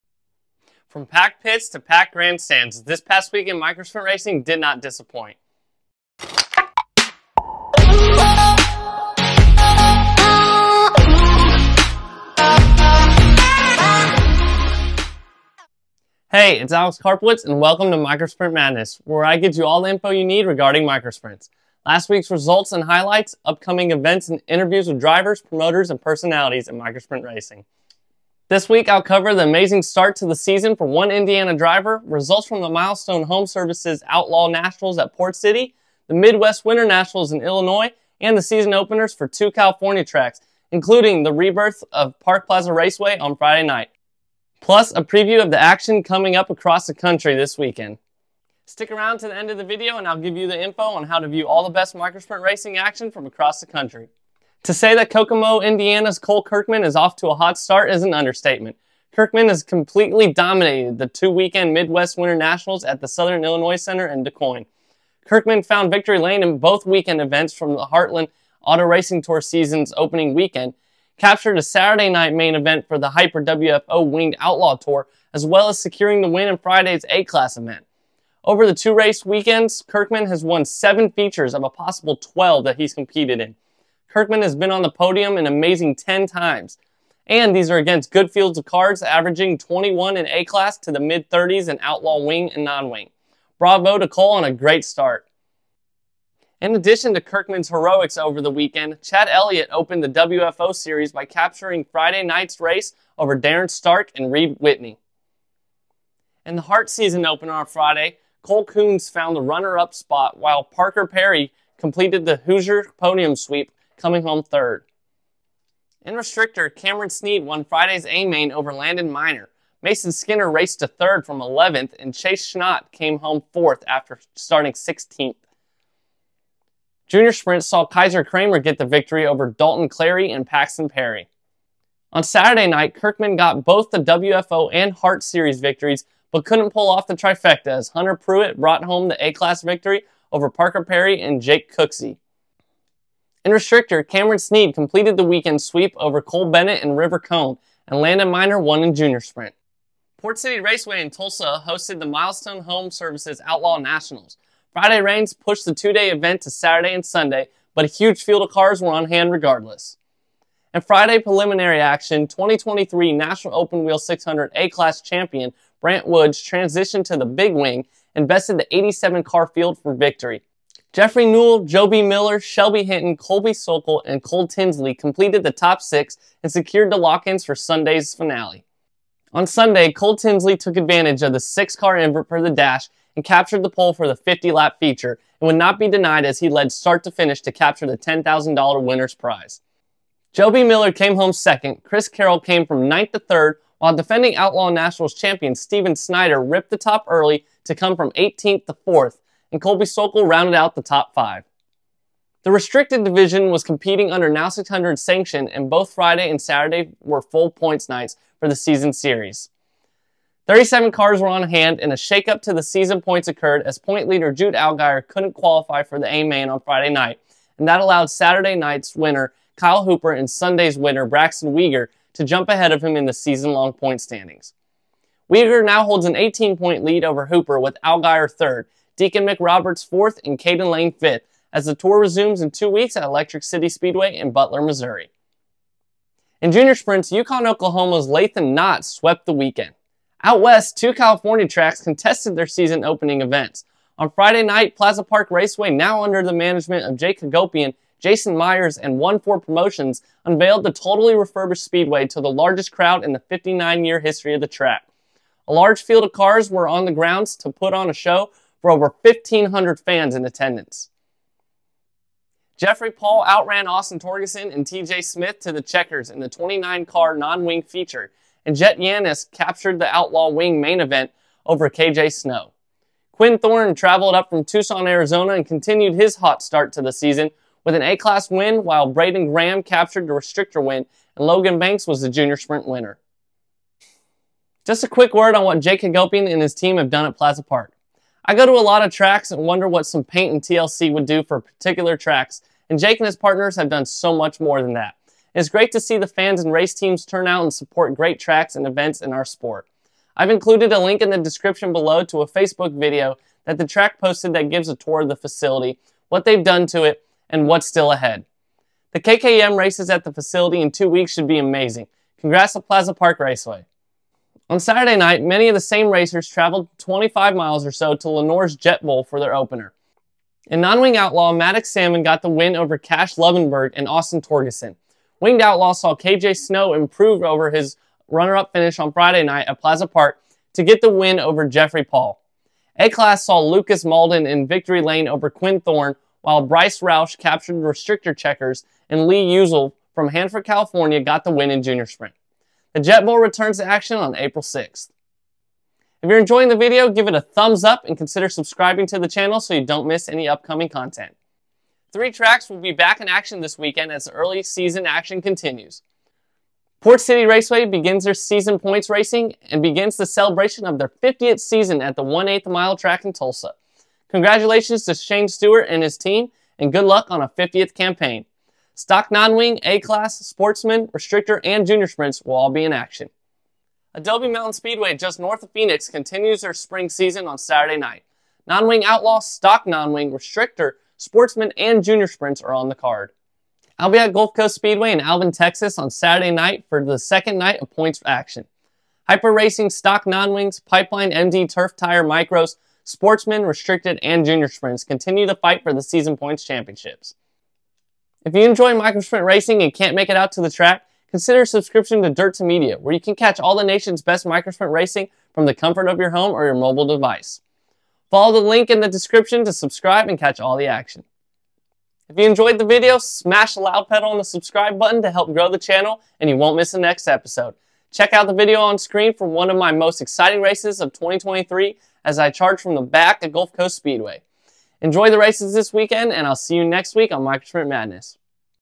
7 wins and $10,000 - An Exciting Weekend of Micro Sprint Racing!
🏁 Welcome to Micro Sprint Madness with Alex Karpowicz! 🏁

🎙️ Buckle up for an exhilarating ride through this past weekend of Micro Sprint racing! In this episode of Micro Sprint Madness, I will take you on a whirlwind tour of the latest racing action from coast to coast. From the dominating performances of drivers like Kole Kirkman in Indiana to the heart-pounding battles at Port City Raceway in Tulsa, and the packed grandstands of Plaza Park Raceway in California, no corner of the Micro Sprint universe is left unexplored. Get insider insights, and all the must-know updates from the track. Whether you're a seasoned fan, driver or new to the sport altogether, this episode promises non-stop excitement and thrills!

📄 Last Weeks Results: Milestone Home Services Outlaw Nationals at Port City, Midwest Winter Nationals in Illinois, and the season openers for two California tracks

📅 Mark Your Calendar: This Weekend - Port City Raceway season points opener, Adobe Mountain Speedway, and Gulf Coast Speedway.

🎥 Can't Make It? No worries!

DIRT2MEDIA

KKM CHALLENGE SERIES
Information and Registration

TIMESTAMPS:
00:00 Intro
00:55 WFO Series and HART Series Results
02:30 Milestone Home Services Outlaw Nationals
04:00 Plaza Park Raceway
05:15 Lemoore Jet bowl
05:50 Upcoming Races This Week
06:38 Dirt2Media